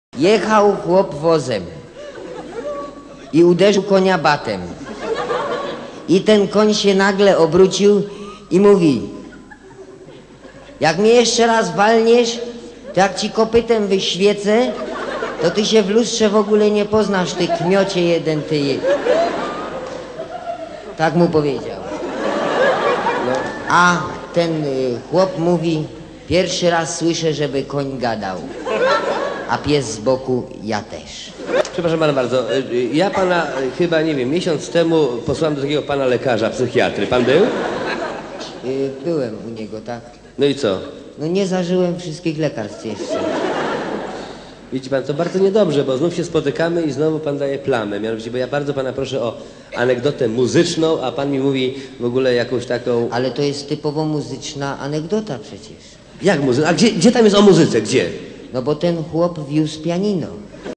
Mustang Parodia - Anegdota Muzyczna
Tytuł mówi za siebie ;)
Mustang: Smoleń i koń
Wartki Potok: Laskowik
Kapitan: chłop
Myślę, że się spodoba XD